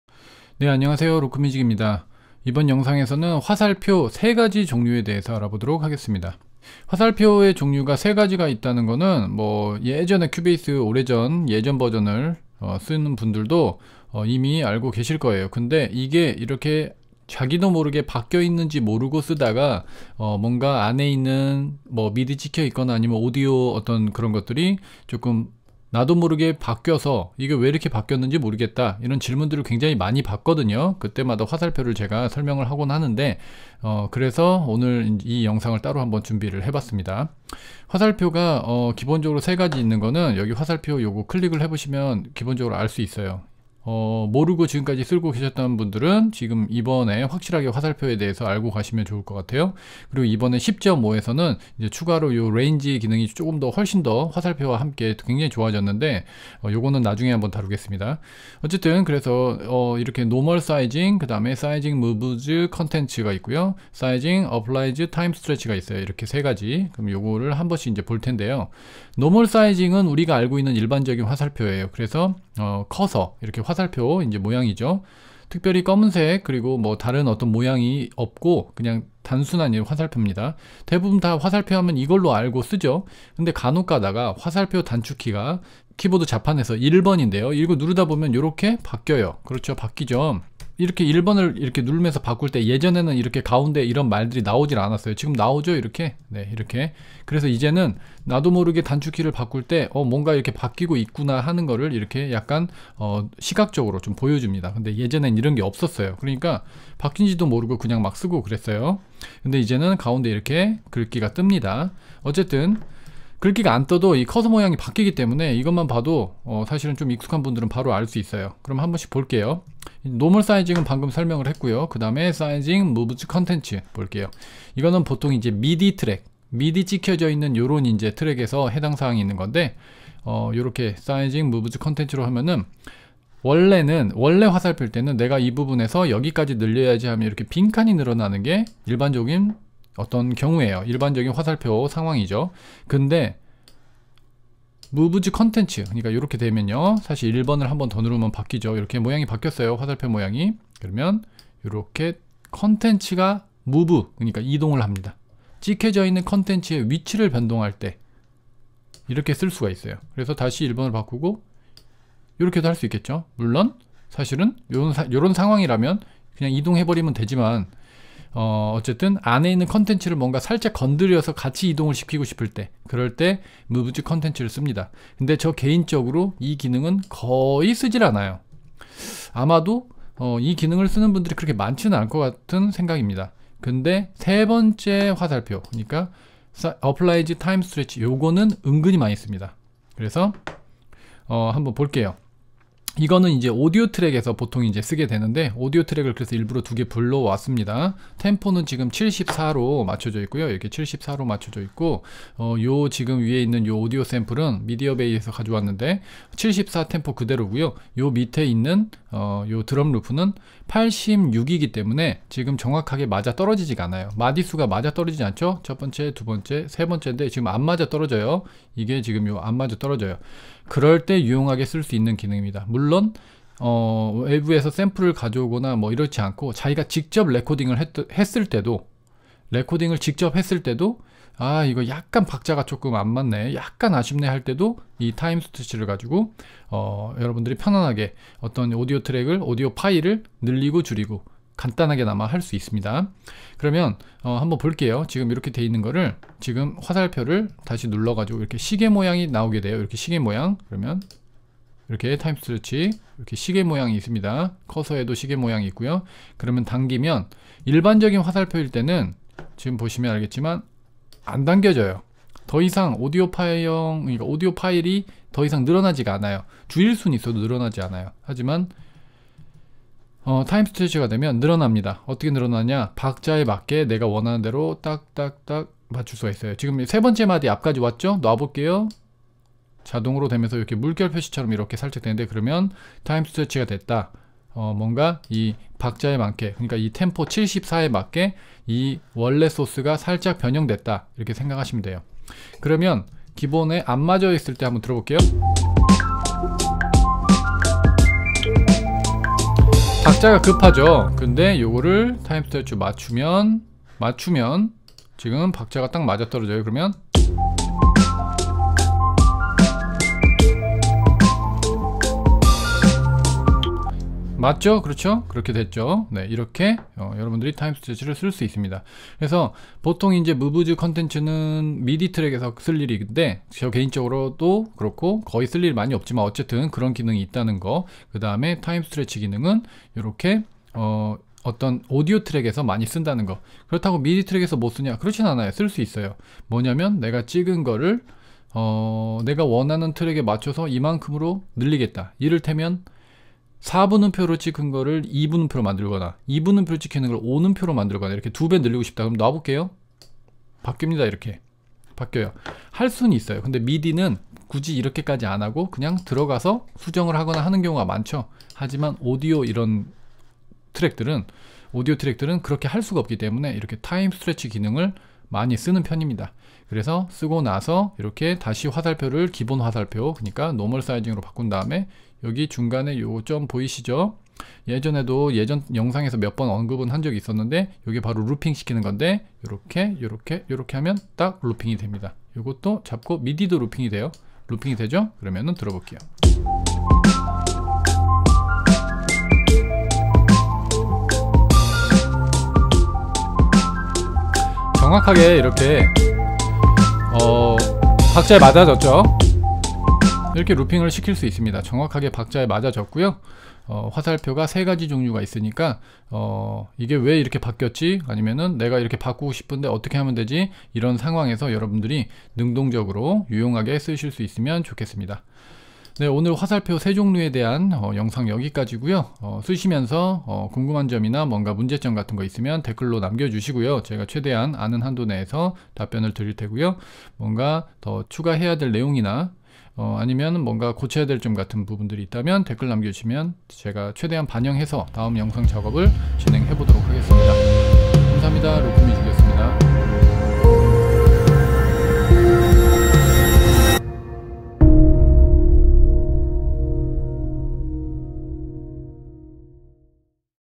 [큐베이스 꿀팁] 화살표 3가지 종류와 기능 (feat. 타임 스트레치)
알아두면 쓸모있는 큐베이스 프로 팁 #28 - 화살표 3가지 종류와 기능 (feat. 타임 스트레치)

: 큐베이스에는 3가지의 화살표 종류가 있습니다. 그에 대해 잘 모르고 단축키 1번을 여러번 누르다보면 화살표가 엉뚱하게 바뀌어서 프로젝트 창의 컨텐츠가 자신도 모르게 변동되는 실수가 발생하거나, 화살표가 자신이 원하는 기능으로 작동하지 않아서 어려움이 생길 때도 있습니다. 하지만 화살표 3가지의 기능을 잘 알게되면 컨텐츠 자체를 이동하거나 타임 스트레치 등의 기능을 자신이 원하는 상황에 맞춰서 바꿔가며 사용할 수 있습니다. 그 3가지의 화살표 종류와 기능에 대해서 알아봅니다.

1. Normal Sizing
2. Sizing Moves Contents
3. Sizing Applies Time Stretch